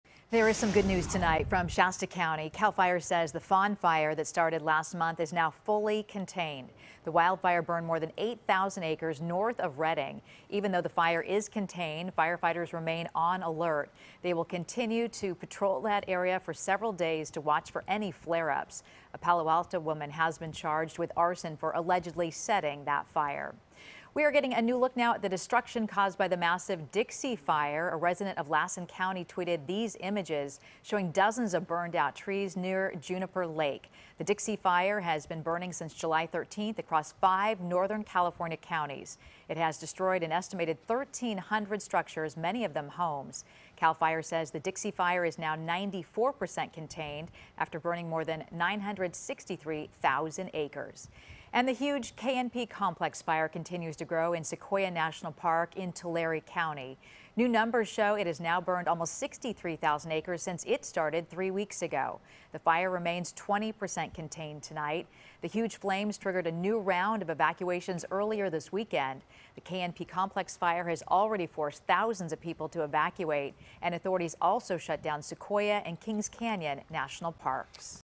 Fawn Fire reaches full containment
Firefighters say the Fawn Fire in Shasta County is fully contained now while the huge Dixie Fire is up to 94% contained. The KNP Comples Fire, however, which has forced the closure of Sequoia National Park continues to be difficult to contain.